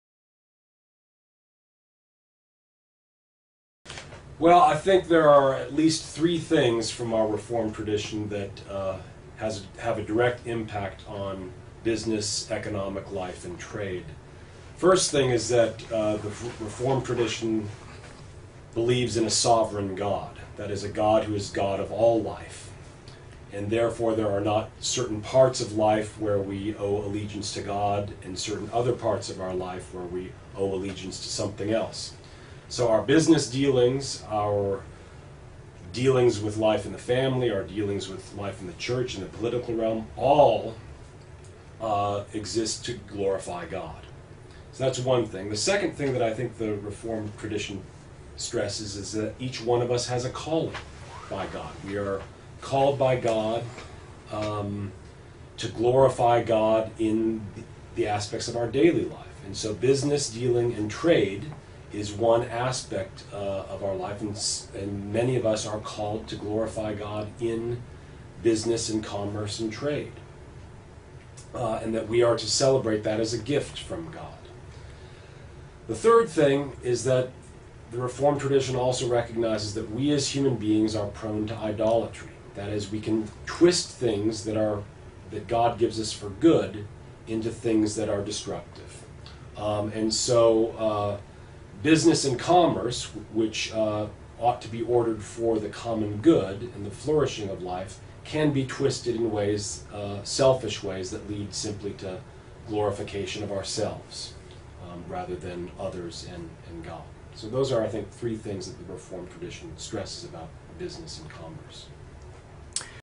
David Jensen - What does the Reformed tradition say about trade? (Part 3)
What does the Reformed tradition says about business and trade? Hear what Austin Presbyterian Theological Seminary scholar David Jensen says about how the Reformed tradition gives us simple principles to evaluate a complex subject like trade.